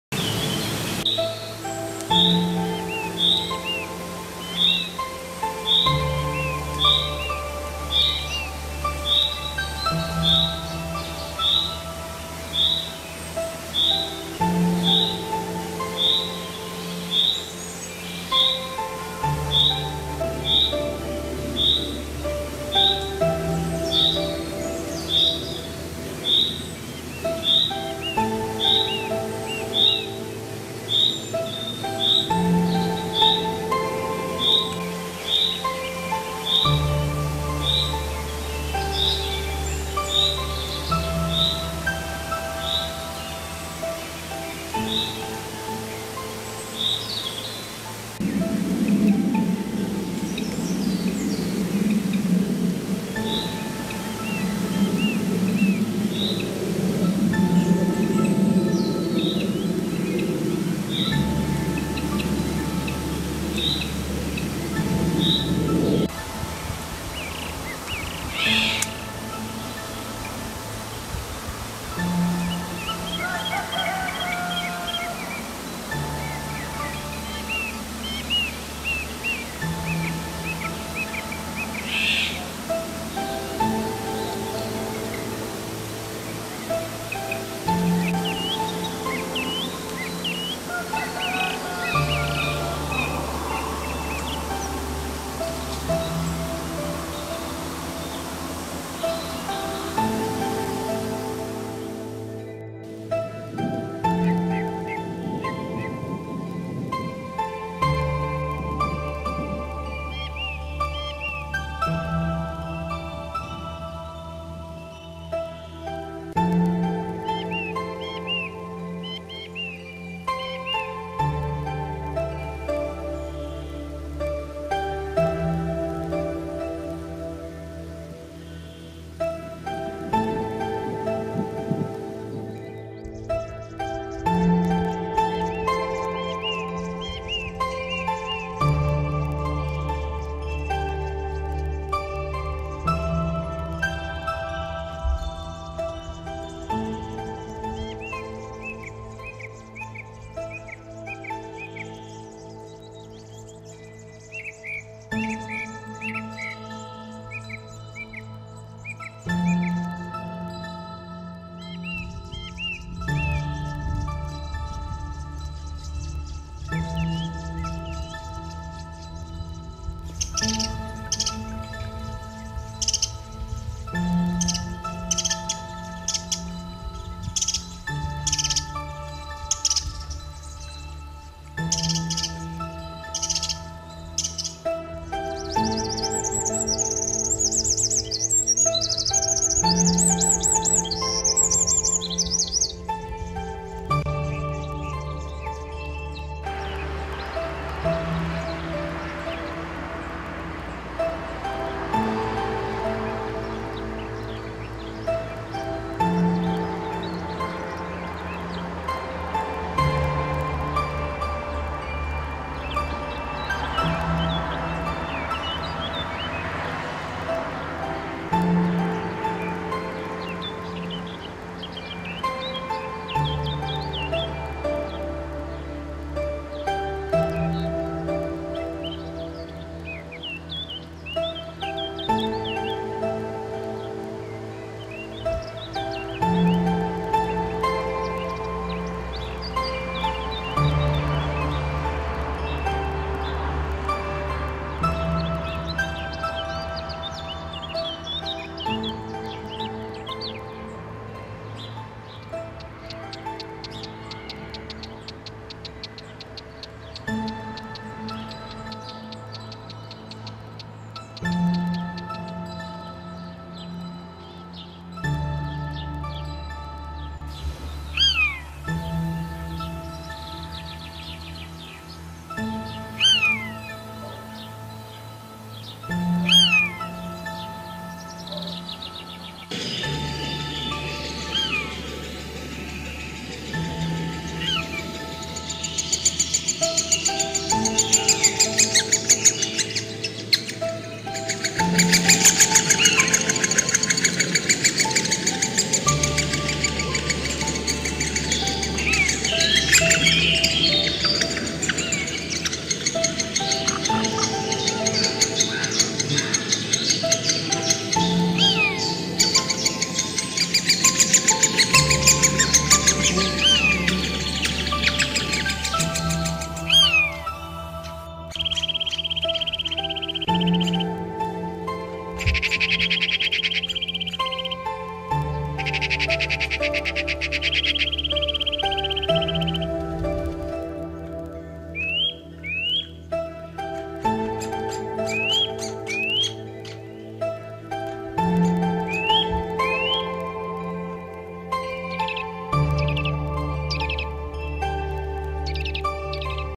Wildlife Video Beautiful Bird Feeding Their Babies #38
Bird
animals and wildlife is beautiful creature on our planet. As you see
they have family to take care
find the food and feed their babies. Most of animal and bird in this video is native Australia and Slovakia wildlife.